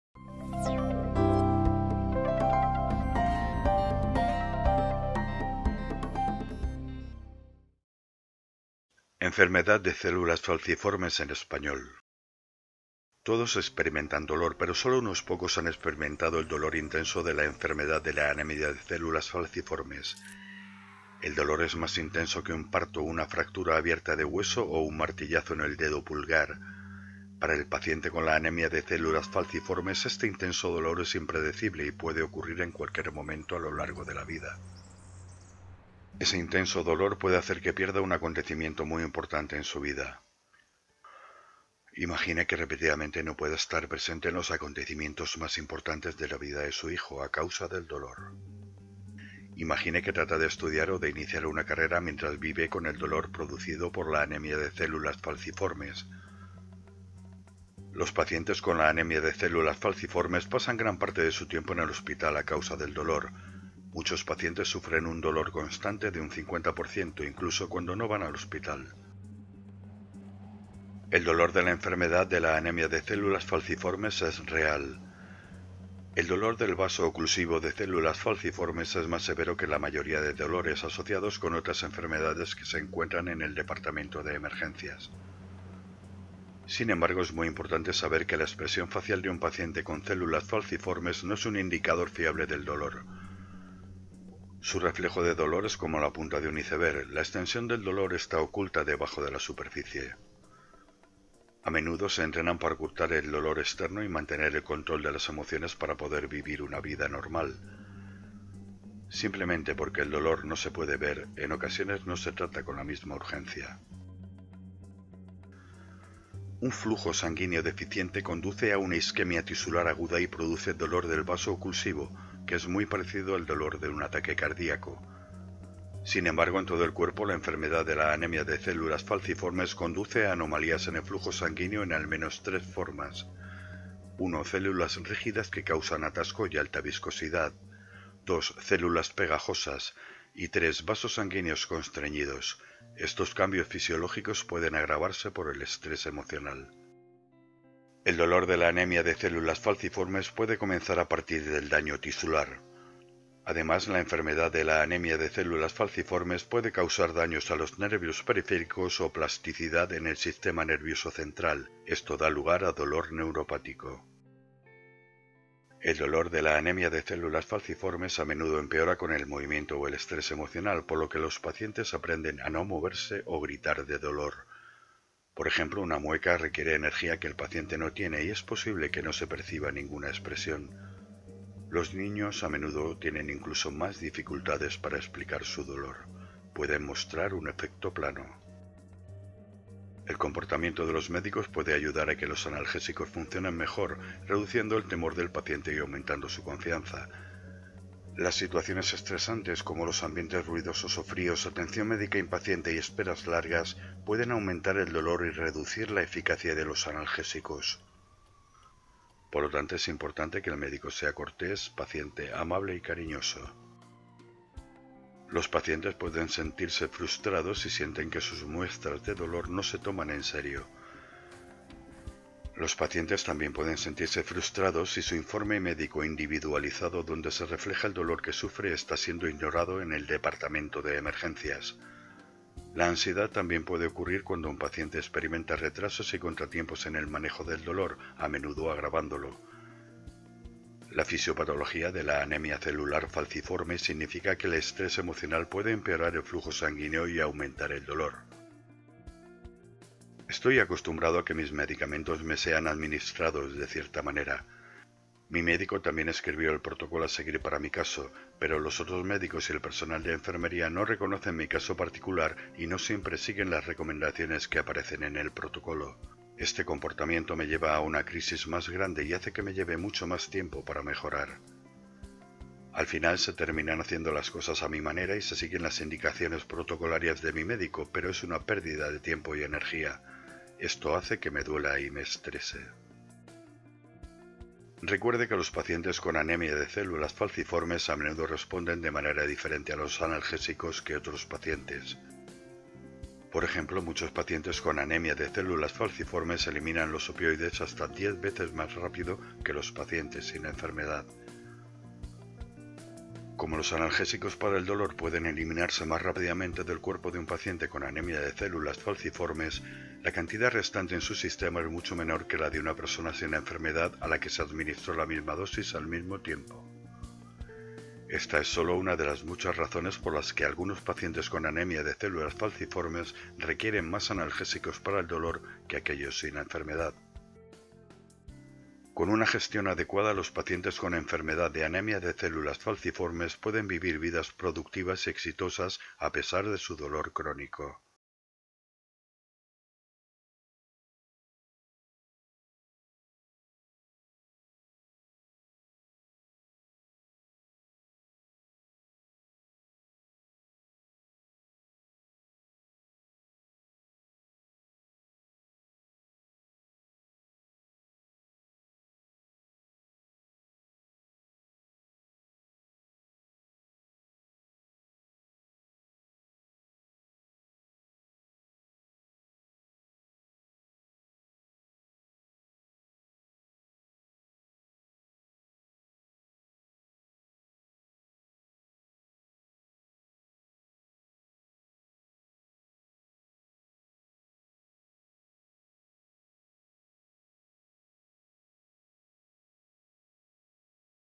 Sickle Cell Disease in Spanish (accent from Spain)
This animation addresses the pain of Sickle Cell disease and how clinicians should handle Sickle Cell patients. The pain of Sickle Cell Disease is very real. Sickle Cell vaso-occlusive pain is more severe than most other pain conditions encountered in the Emergency Department. However, it is very important to know that the facial expression of a Sickle Cell patient is not a reliable indicator of pain. They often train themselves to hide the outward pain and keep control of emotions, in order to live with many days and years of pain. Just because it cannot be seen, it is sometimes not treated with the same urgency. Clinicians' behaviors can help the pain medications work better by reducing the patient's fear and increasing their trust. Thus, it is important for the clinician to be courteous, patient, gentle and caring. With adequate management, patients with Sickle Cell Disease can live productive, successful lives despite their chronic pain.